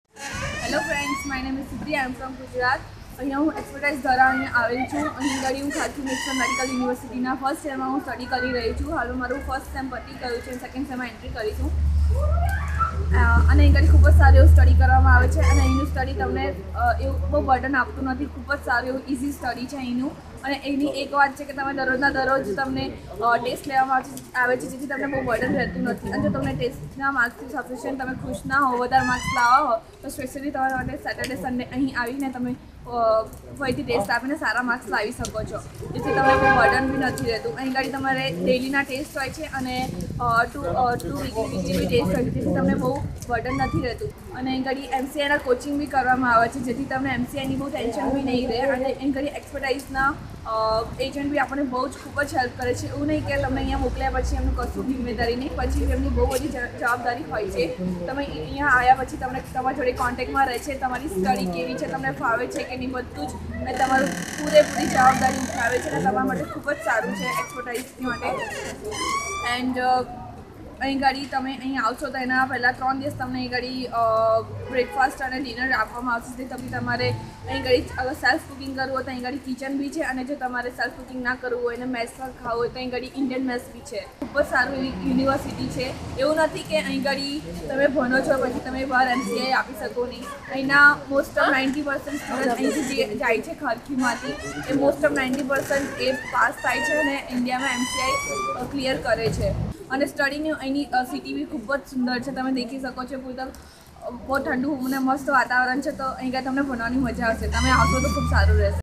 Girl from Gujarat, studying mbbs in Ukraine|| why to choose Ukraine
Students from Xpertise education Consultancy